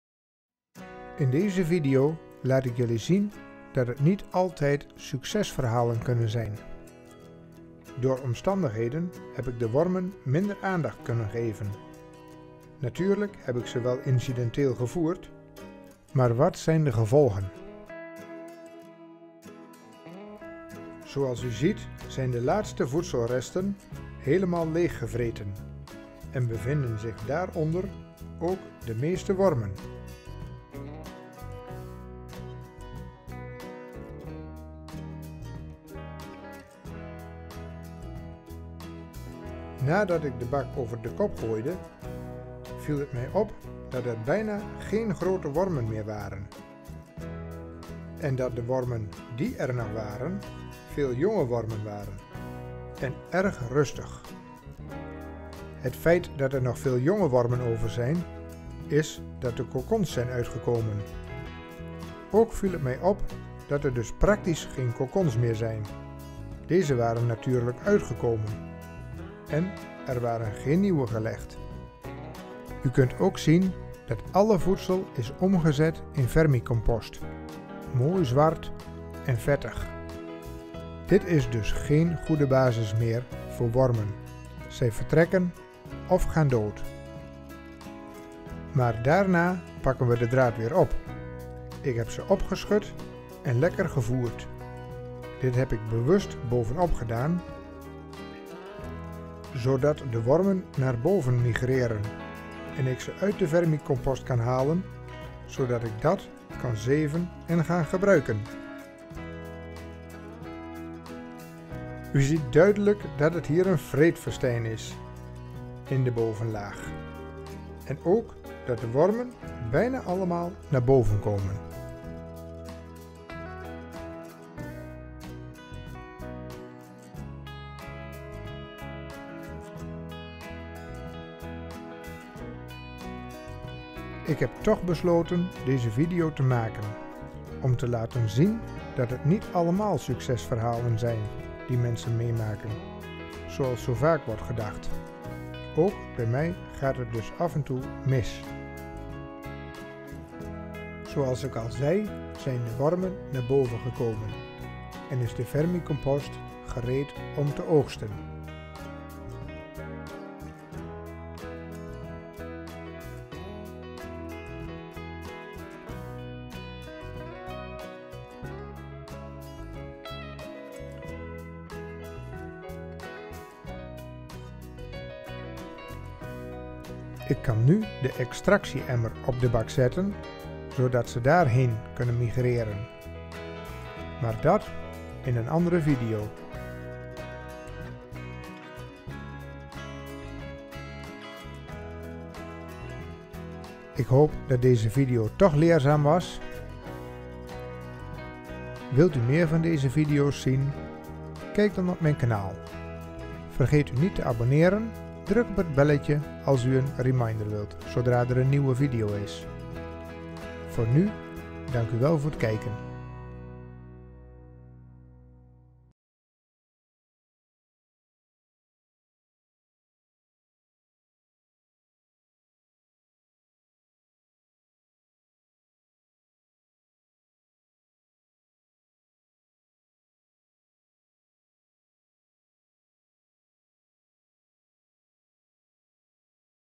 Niet altijd succes, mijn realiteit...
Deze video heb ik gemaakt om te laten zien dat er ook bij mij wel eens iets fout gaat.